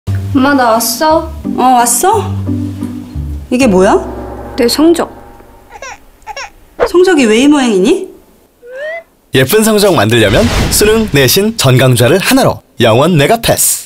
[메가스터디] NEW 0원 메가패스가 온다! (feat. 너 성적이 왜 이 모양이니?)
너 성적이 왜 이 모양이늬???🤮
예쁜 성적 만들고파? (feat.미란이언니❣)

✔11월 11일 11시 11분
✔예쁜 성적 만들어 줄
✔첫, 시작, 1등, 단 하나의 패스
✔NEW 0원 메가패스가 온다✨✨✨

커밍쑤운 🔥
미란언니의 예쁜 성적 만들기 풀영상도 함께 공개❣